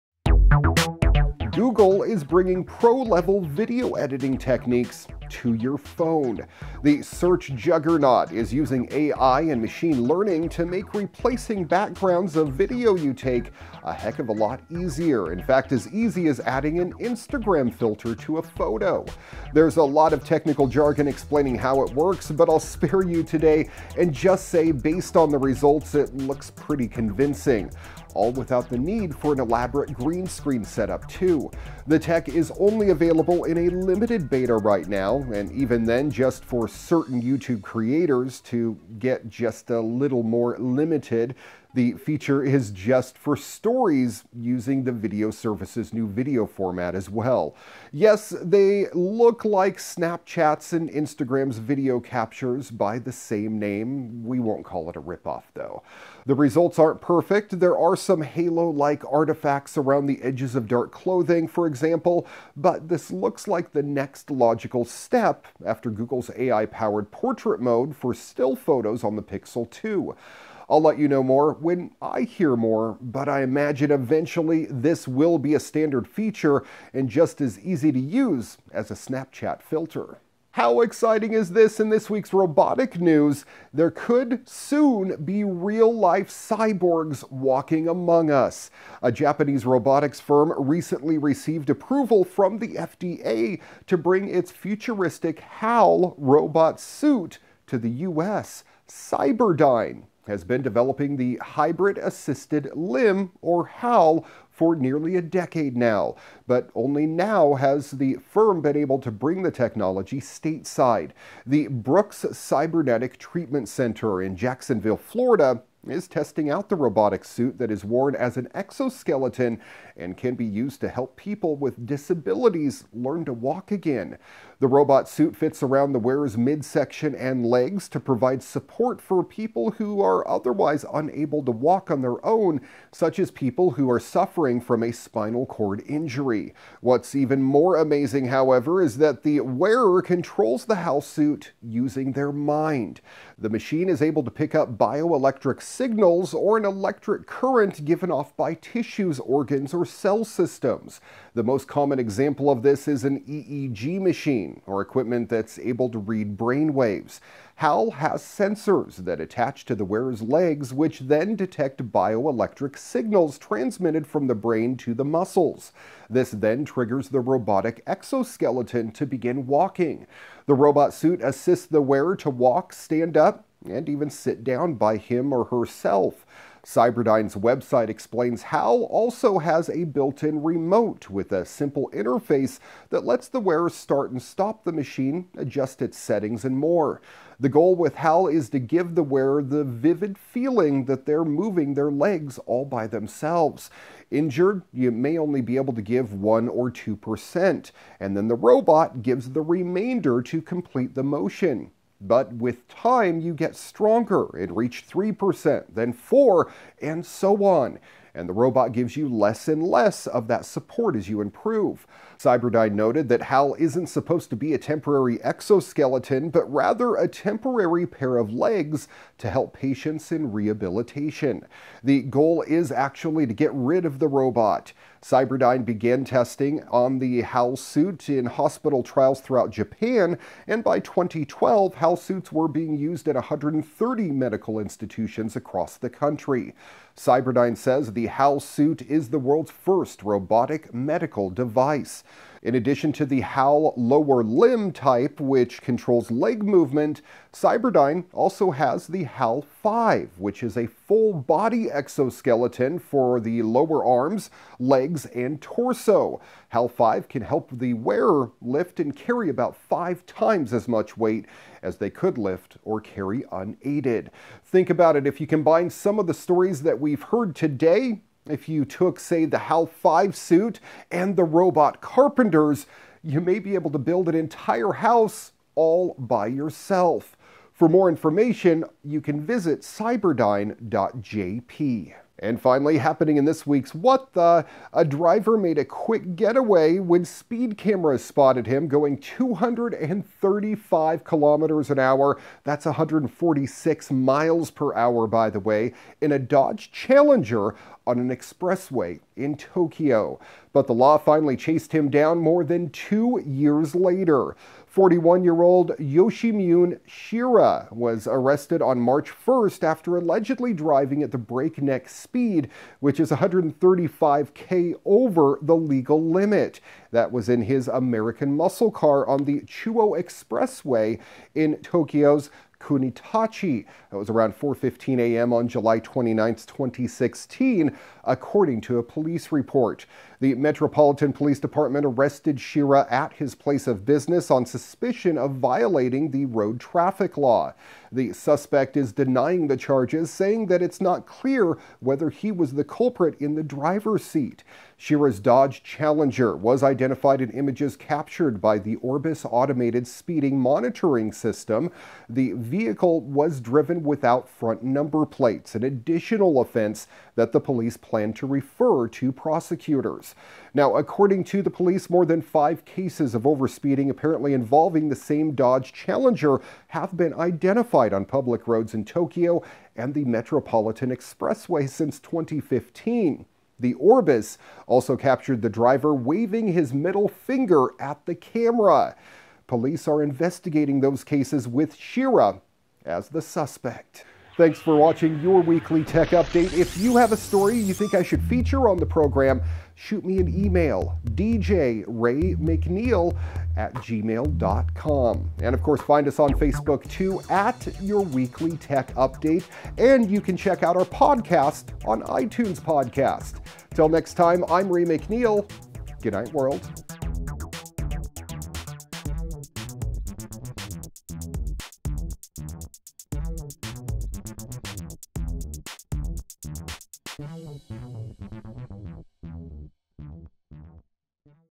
Your Weekly Tech Update EP. 52 Part 3 of 3: Cyberdyne's Cyborg legs. - What The...???
A Live-action 'Far Cry 5' short film heads to Amazon Prime.

Cyberdyne's Cyborg legs' could help people learn to walk again after spinal injuries.

And happening in this weeks What The…???

A Man’s 235 kph expressway ride catches up with him 2 years later.